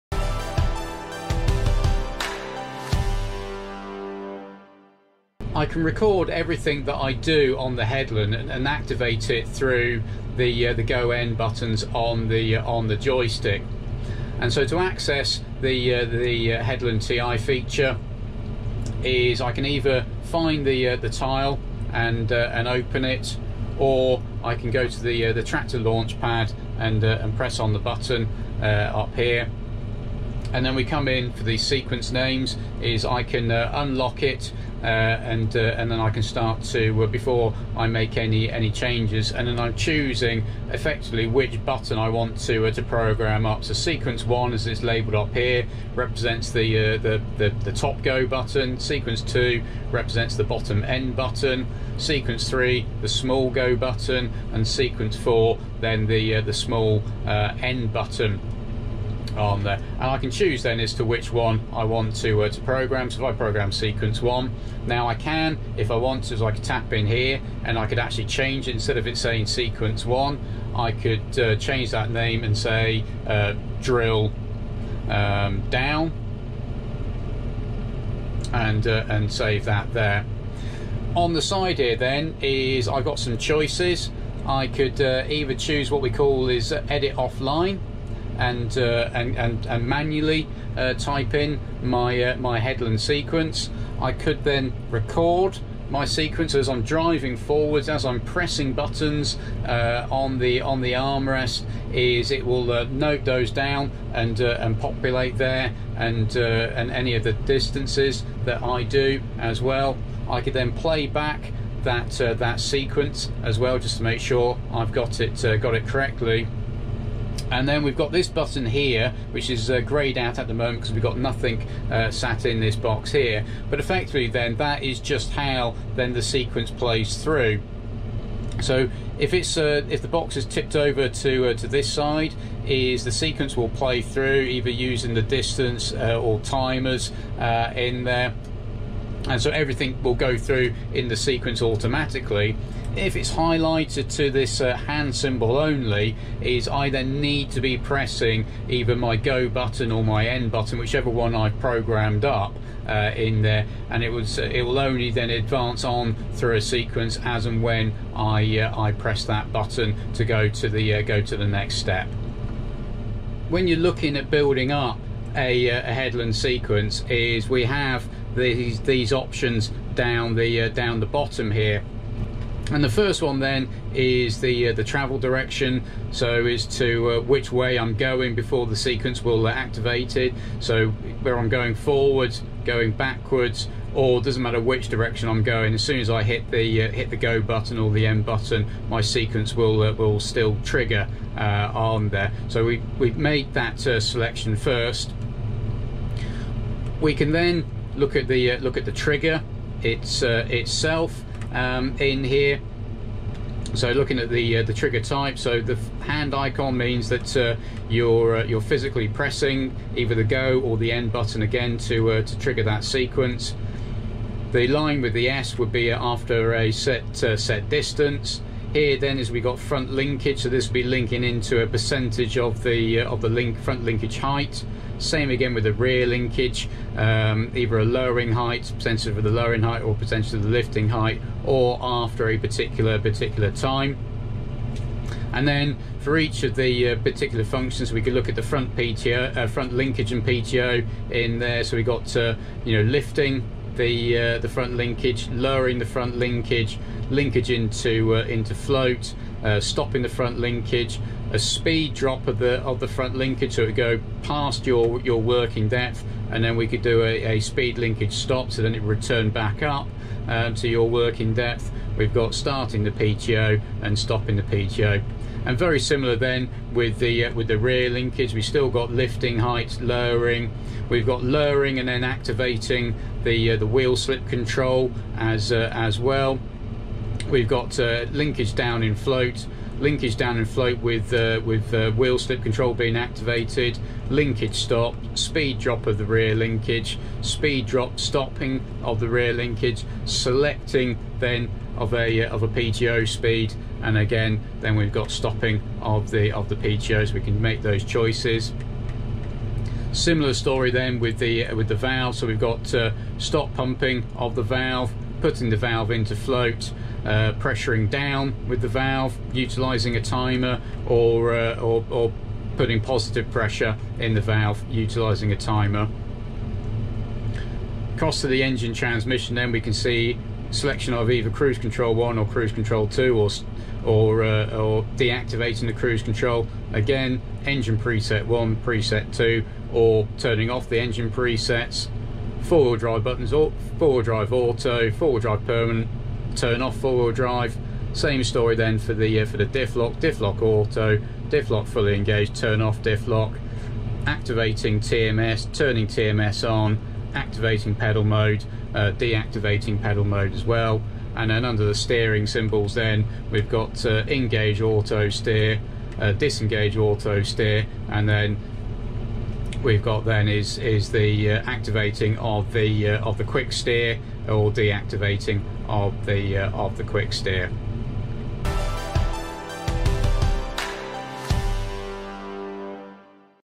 FendtOne TeachIN explained
Explanation of the the buttons and options for the Headland TeachIN using the FendtOne terminal